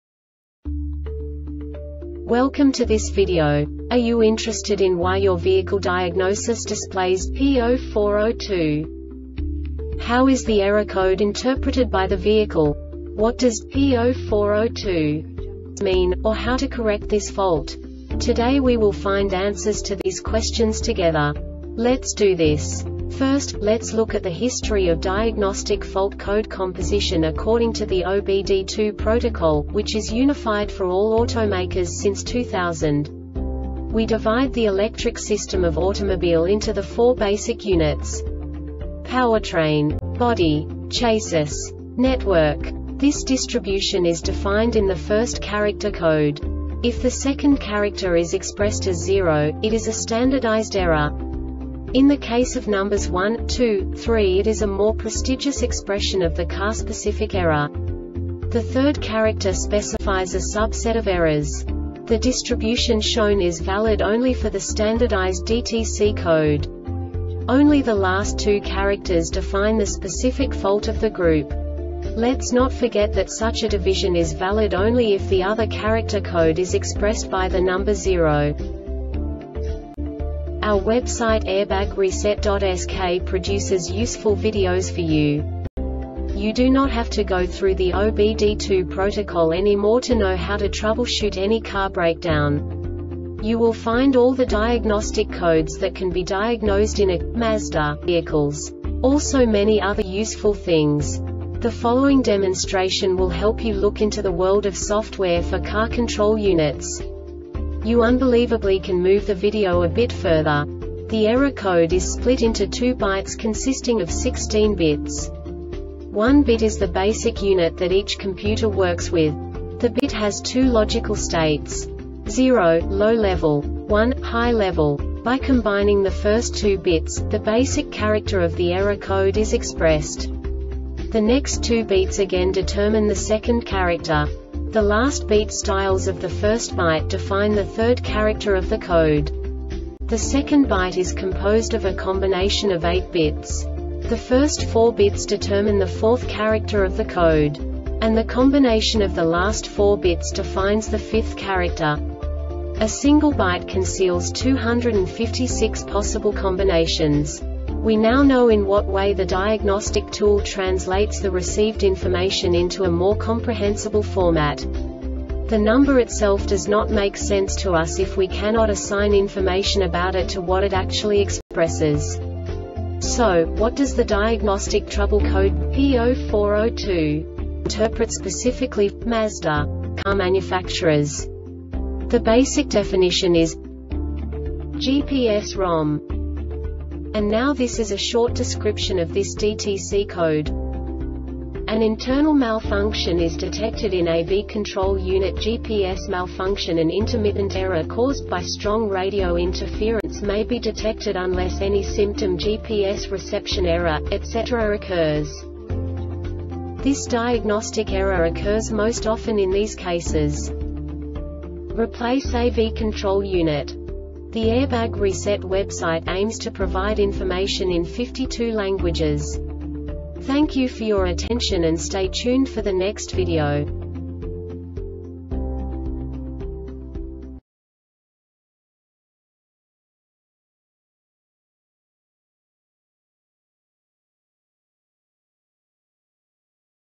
DTC Mazda P0402 Short Explanation
Contents:
0:21 Basic DTC analysis according to OBD2 protocol standard.
1:48 Insight into programming
2:58 A diagnostically understandable description of the Diagnostic Trouble Code
3:05 Basic error definition

Make:
Mazda
Code:
P0402
Definition:
GPS ROM
Description:
An internal malfunction is detected in AV control unit (GPS malfunction).An intermittent error caused by strong radio interference may be detected unless any symptom (GPS reception error,etc. ) occurs.
Cause:
Replace AV control unit.